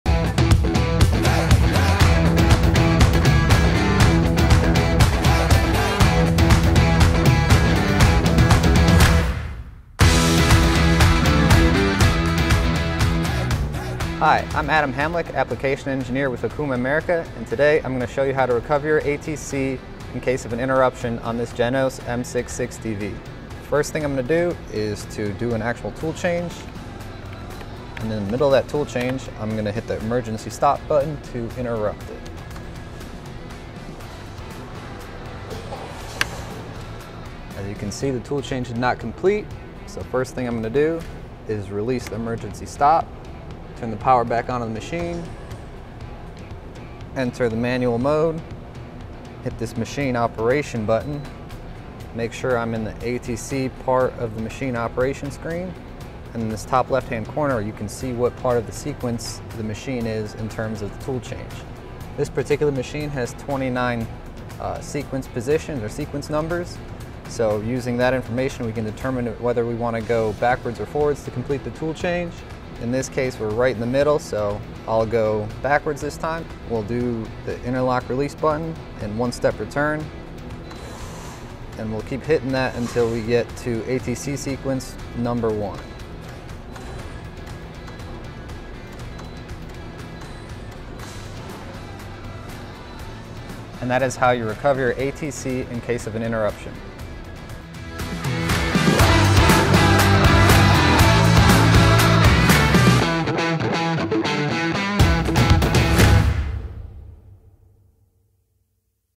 Tech Tip: How to Perform Automatic Tool Changer (ATC) Recovery
Don’t let an interruption derail your machining process - discover how to recover your automatic tool changer (ATC) in case of an unexpected interruption.